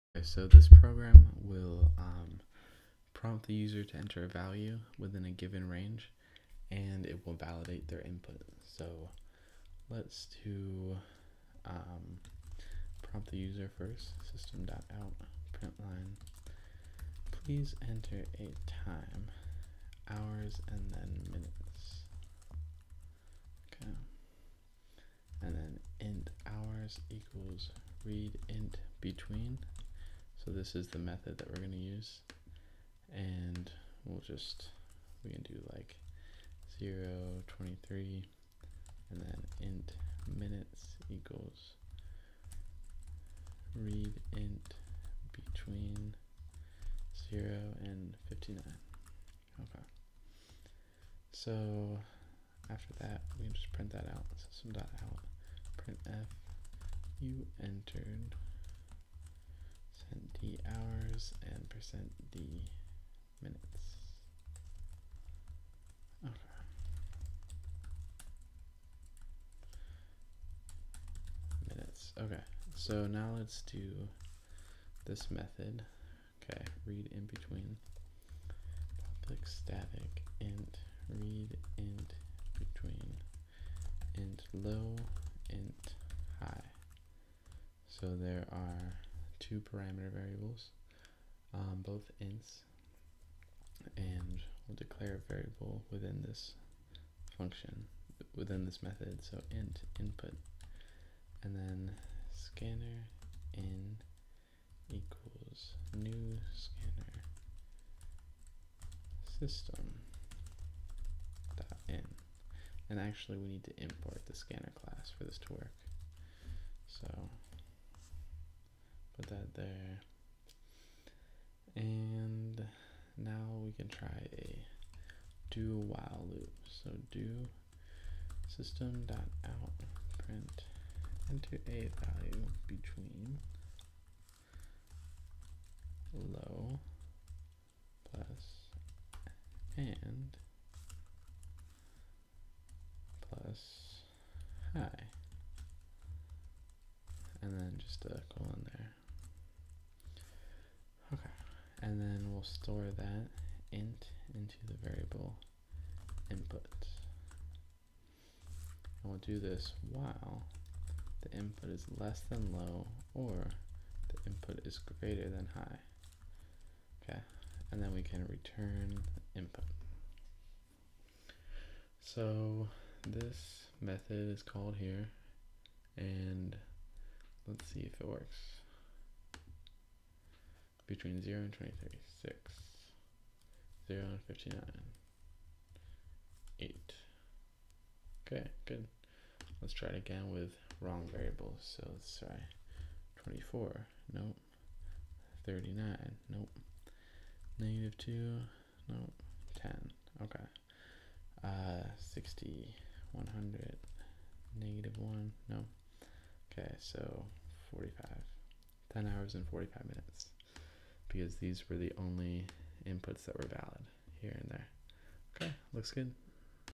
Validate Input Within a Range of Numbers
This program prompts the user until their input is within a certain range of numbers.